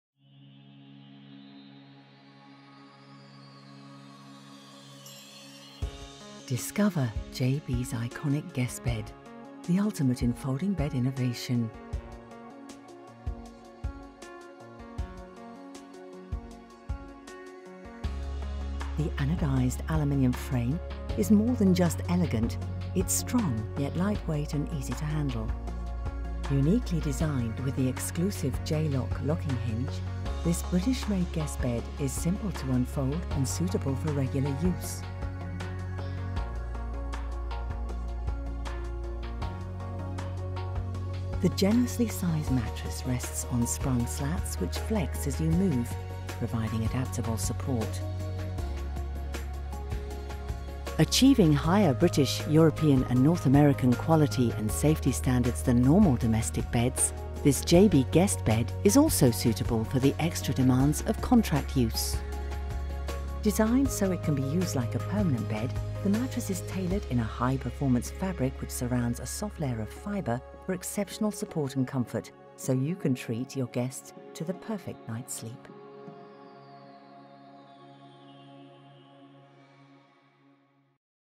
jaybe folding bed with aluminium frame performance mattress
Created without compromise, made using only the finest materials, the anodised aluminium frame is lightweight and rests on high quality 360° castors so you can manoeuvre and operate it easily. Precision injection moulded components compliment the stylish appearance and the exclusive J-Lok® folding mechanism enables you to open and close it effortlessly - one side at a time.

Tailored in a high performance fabric for greater durability, the two-metre length mattress combines a top layer of soft fibre with a higher density base layer for exceptional support and comfort. This Jay-Be guest bed also achieves higher British, European and International performance standards than normal domestic beds making it suitable for the extra demands of contract use.